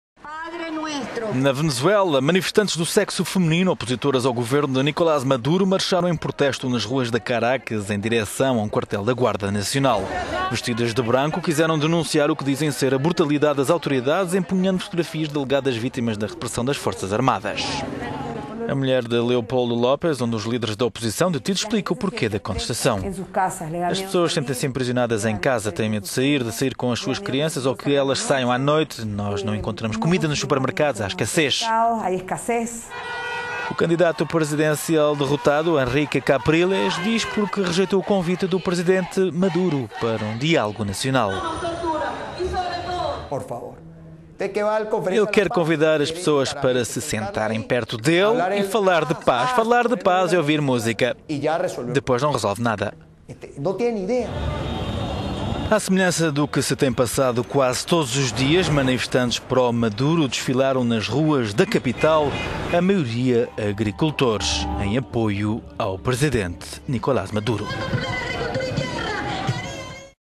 Venezuela: Mulheres opositoras ao governo denunciam "brutalidade" das autoridades
Manifestantes do sexo feminino, opositoras ao governo de Nicolás Maduro marcharam em protesto nas ruas de Caracas, na Venezuela, até um quartel da Guarda Nacional.

Vestidas de branco, quiseram denunciar o que dizem ser a brutalidade das autoridades, empunhando fotogradias de alegadas vítimas da repressão das forças de segurança.

A mulher de Leopoldo Lopez, um dos líderes da oposição, detido, explica o porquê da contestação. "As pessoas sentem-se emprisionadas em casa, têm medo de sair, de sai...